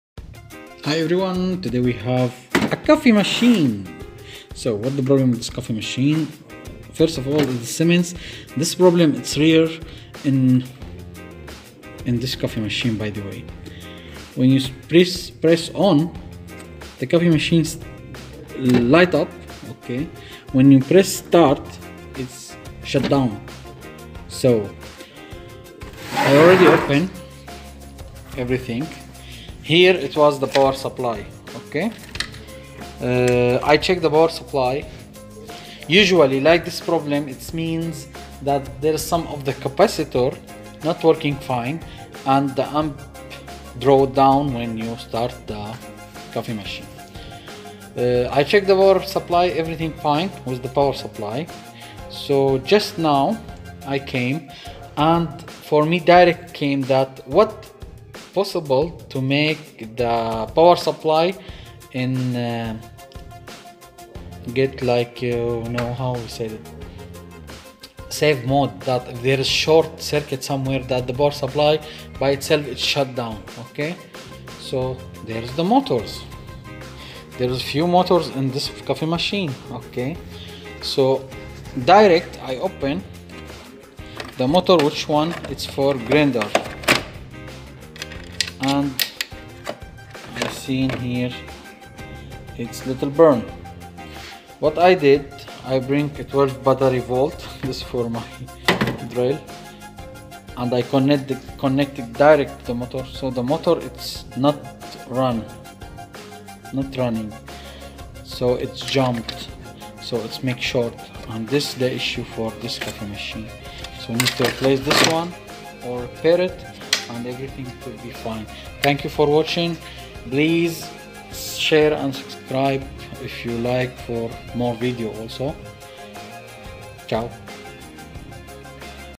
SIEMENS coffee maker TK76K572 . Power problem get on and shutdown after start processing
Motor shorted to ground